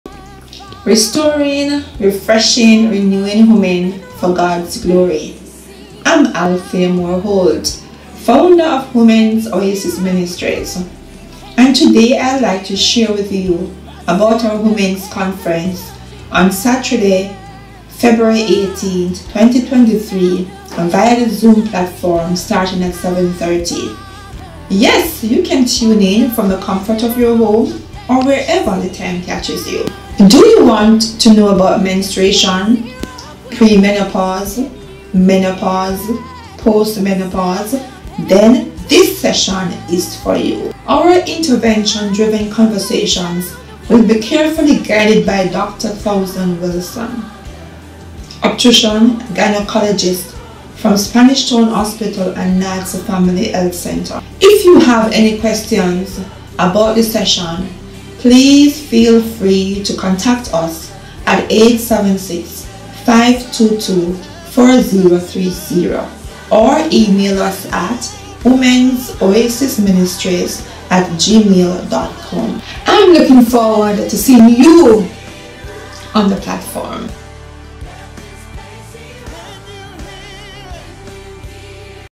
Menstruation and Menopause - Saturday February 18, 2023 @ 7:30 p.m. via zoom platform
Intervention driven conversation with Dr. Townsend-Wilson on menstruation, pre-menopause, menopause and post-menopause